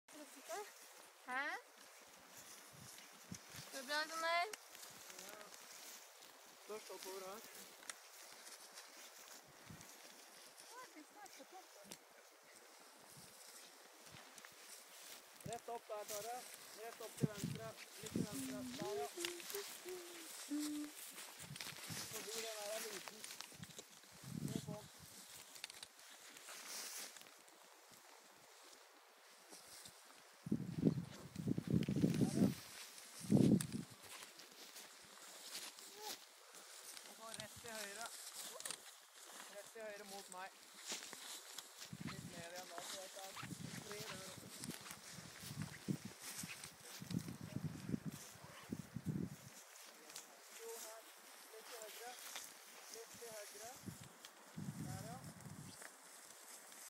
Jenter med krykker og kunstige bein
De kommer seg fram i tett krattskog, mens det blinker metall i kunstige leggbein. Ingen grenser-jentene er tøffe!